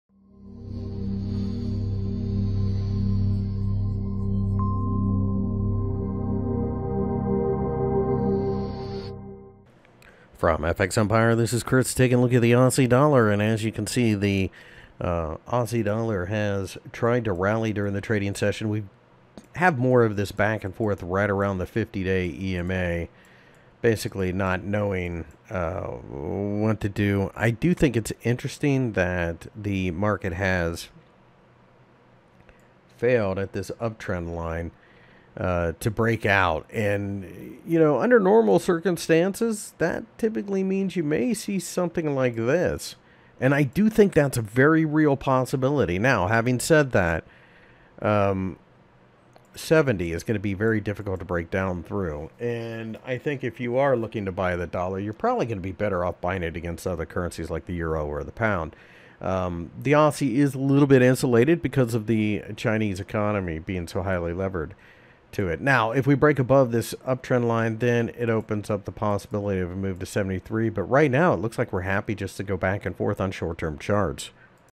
AUD/USD Technical Analysis for October 7, 2020 by FXEmpire
The Australian dollar went back and forth during the trading session again on Tuesday as we continue to go back and forth around the 50 day EMA. Ultimately, this is a market that is currently trying to break above a previous uptrend line.